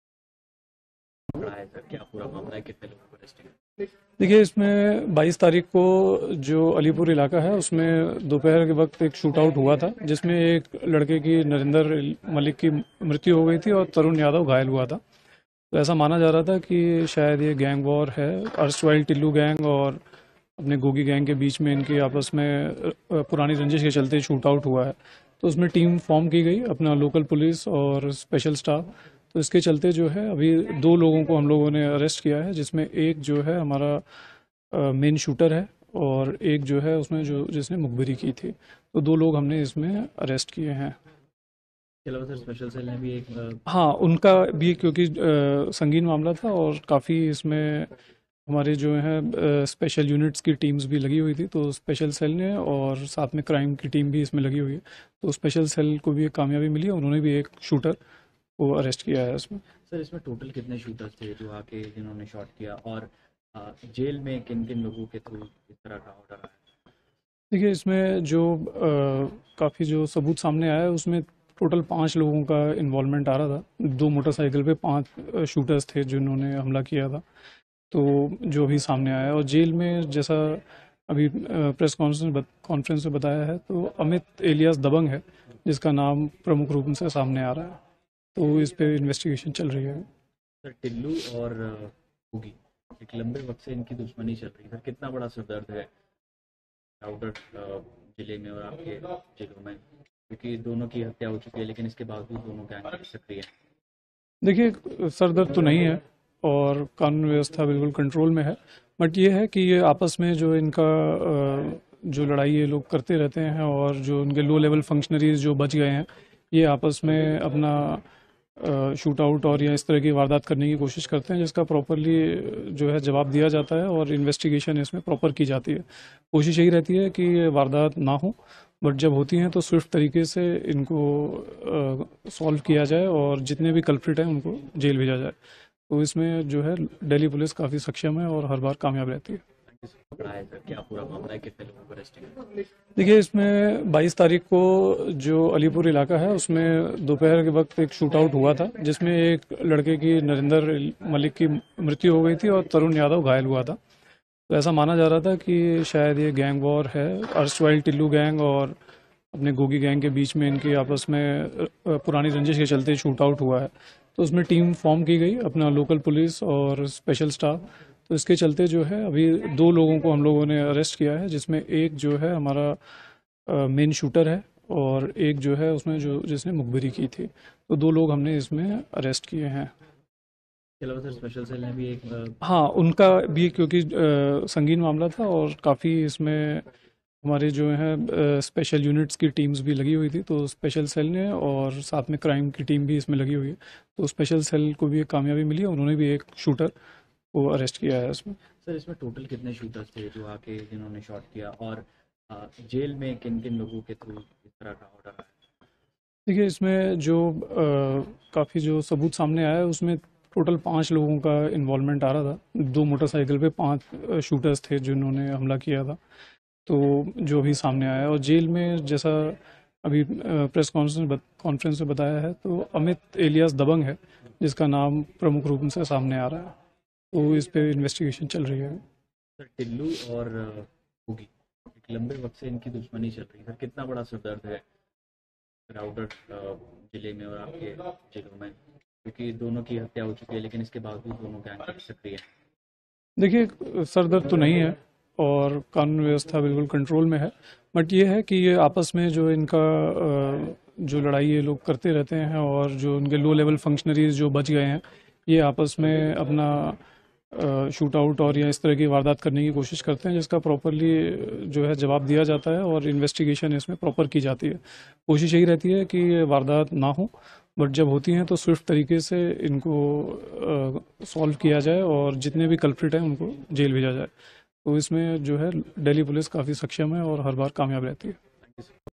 दिल्ली पुलिस का बड़ा खुलासा।। अलीपुर मे हुई गैंगवार मे दो आरोपी गिफ्तार। DELHI POLICE।।
SAMAJ JAGRAN हिंदी न्यूज चैनल एवं समाचार पत्र।।